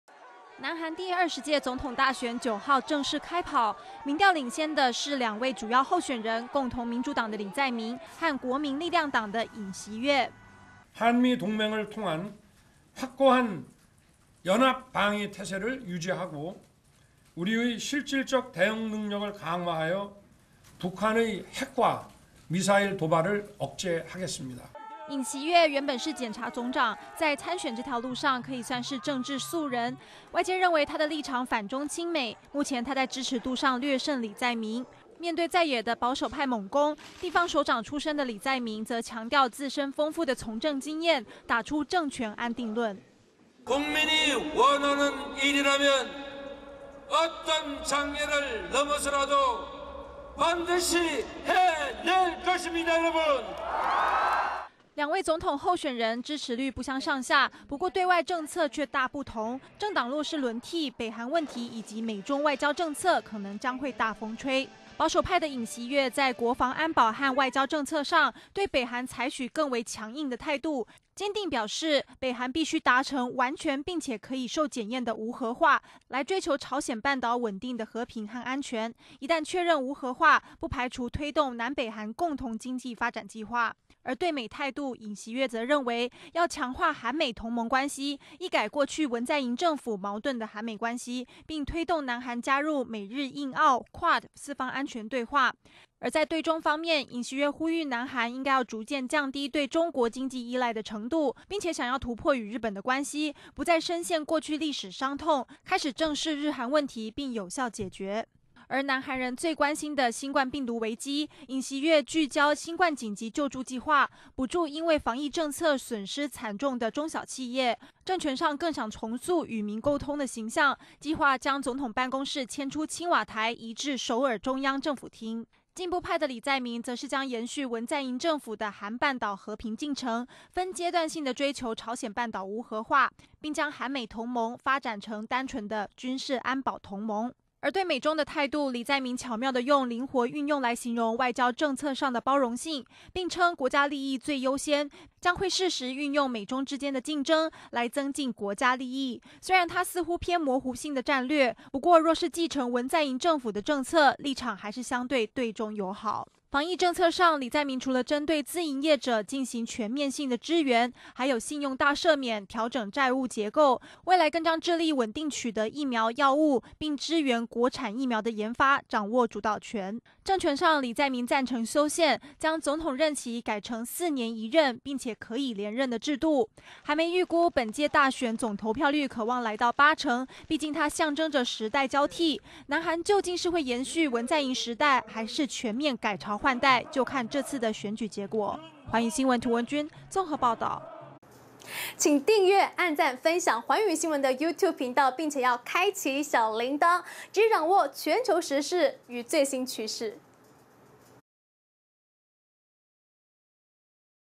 南韓總統大選！尹錫悅.李在明兩強對決
今天就是南韓總統大選的正式投票日，這次選情膠著，執政黨共同民主黨候選人李在明，對上最大在野黨國民力量黨候選人尹錫悅，兩人不但支持度差不多，連之前爆出的醜聞，份量也都差不多。不過雙方的內政外交政策方針大有不同，因此備受關注。估計選舉結果，要到10日早上，才有可能明朗。